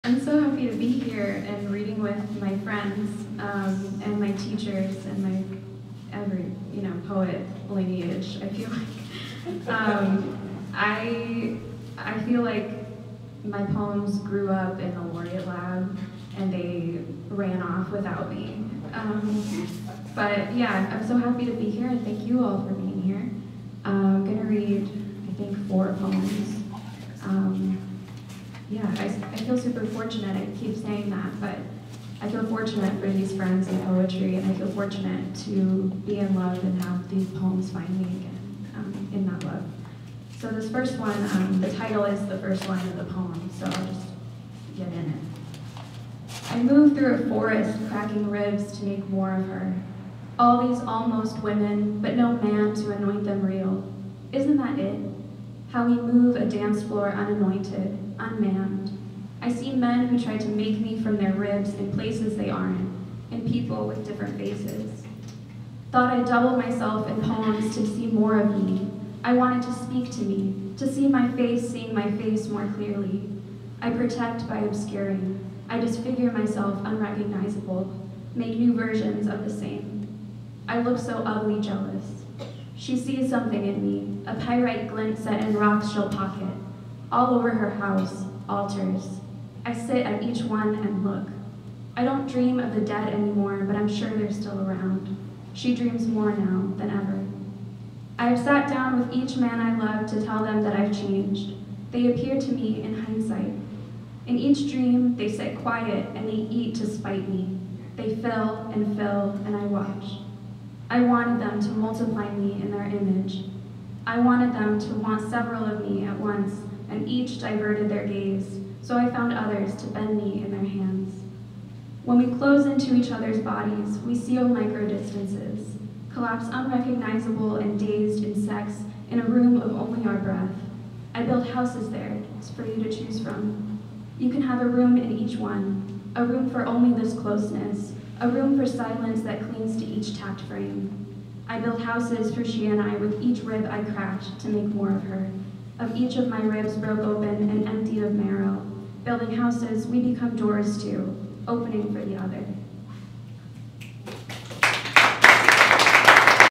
Maria Bosch - LitHop 2021
October 2021 - Mariah Bosch performs at LitHop Fresno, as part of a series of shows featuring performers from the Laureate Lab Visual Wordist Studio. Recorded in the Vista Theatre, inside the Fresno Music Academy, in Fresno's Tower District.